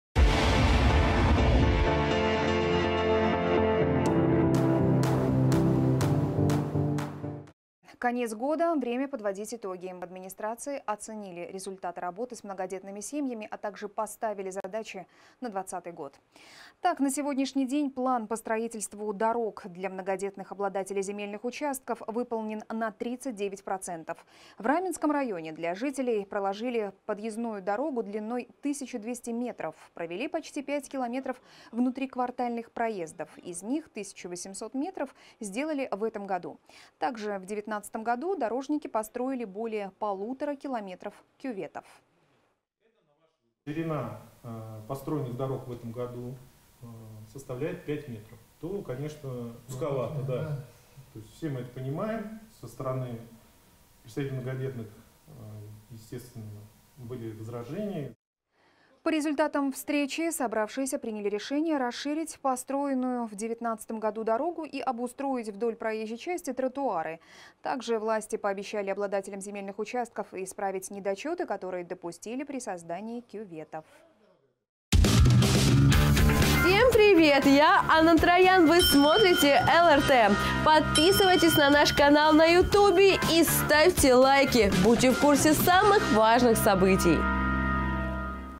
Многодетные семьи обсудили итоги года с властью
Конец года – время подводить итоги. В администрации оценили результаты работы с многодетными семьями, а также поставили задачи на 2020-й год.

Так, на сегодняшний день план по строительству дорог для многодетных обладателей земельных участков выполнен на 39%. В Раменском районе для жителей проложили подъездную дорогу длиной 1200 метров, провели почти 5 км внутриквартальных проездов, из них 1800 метров сделали в этом году. Также в 2019-м дорожники построили более полутора километров кюветов.

Николай Дымов, начальник управления имущественным комплексом администрации г.о. Люберцы:
"Ширина построенных в этом году дорог – 5 метров. Это узко, мы это понимаем. Со стороны многодетных были возражения.."

По результатам встречи собравшиеся приняли решение расширить построенную в 2019-м дорогу,  и обустроить вдоль проезжей части тротуары. Также власти пообещали обладателям земельных участков исправить недочеты, которые допустили при создании кюветов.

Анна Троян, Александр Моисеев.